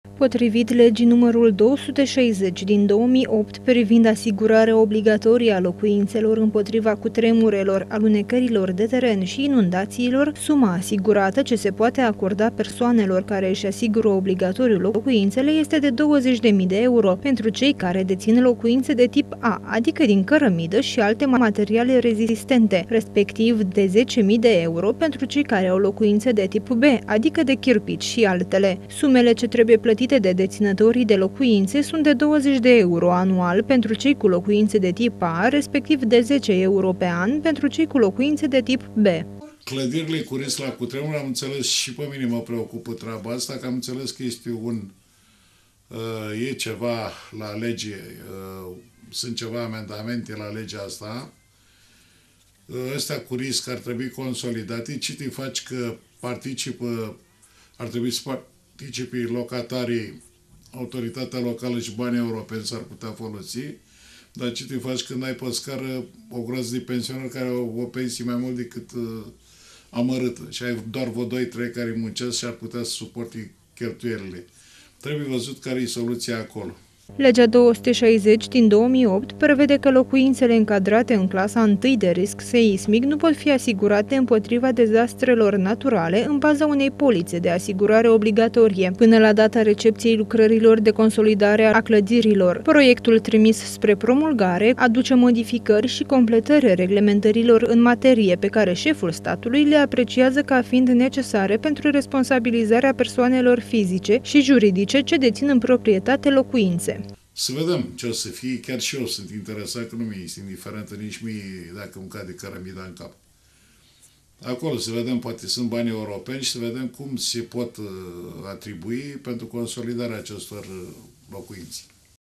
Ştire Etv - Asigurarea în caz de cutremur | 01.05.2015
Legea nr.260/2008 privind asigurarea obligatorie a locuinţelor împotriva cutremurelor, alunecărilor de teren şi inundaţiilor conţine articole contradictorii. Din acest motiv, preşedintele României a trimis spre reexaminare legea, în Parlament. Proiectul trebuie completat pentru a îmbunătăţi cadrul de funcţionare a sistemului de asigurare, întrucât, un articol prevede că cele mai vulnerabile proprietăţi nu pot fi asigurate.